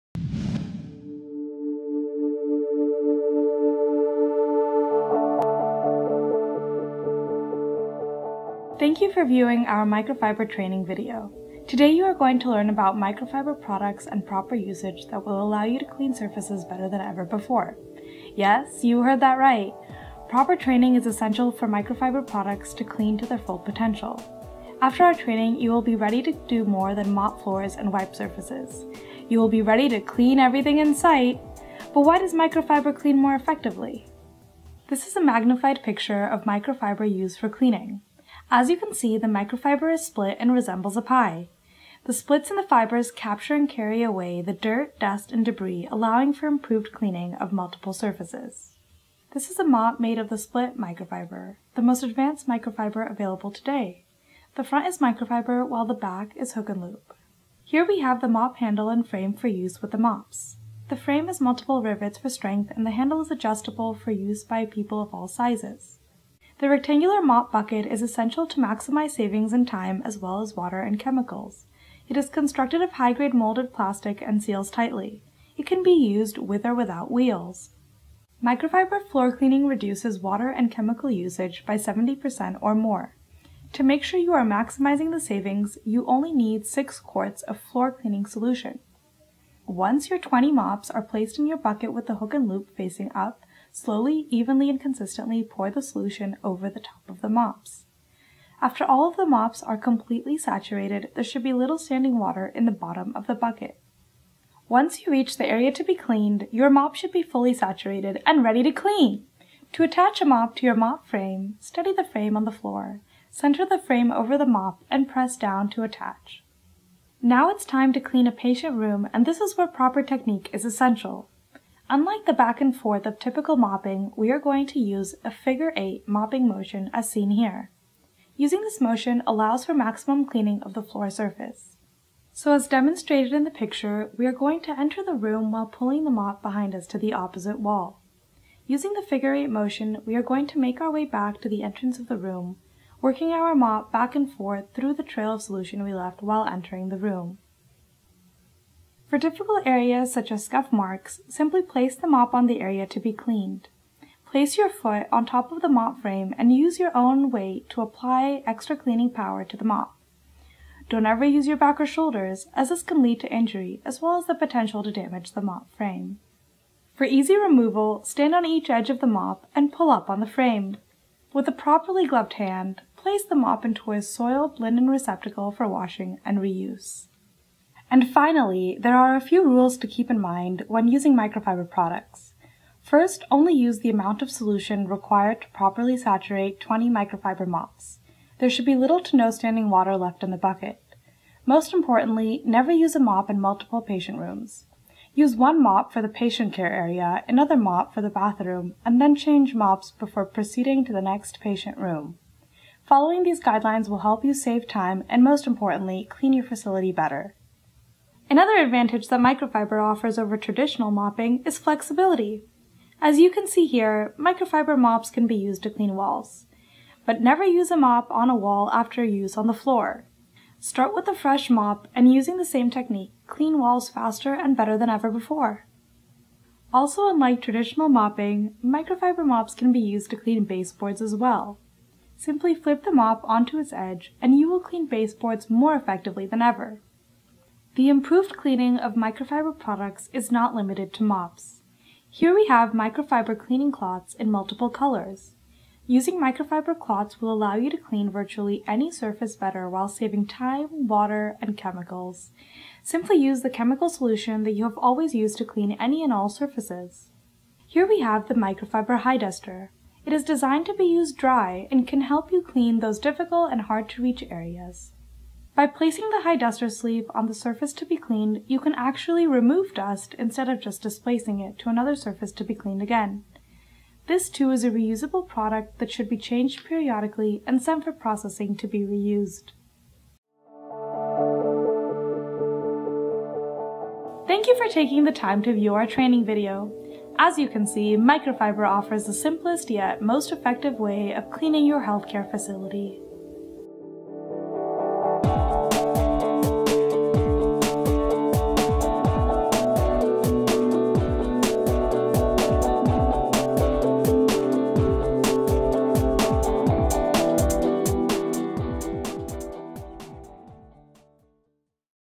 Leading Edge Products In Service Training Video ENGLISH
Proper Cleaning Procedure using Leading Edge Microfiber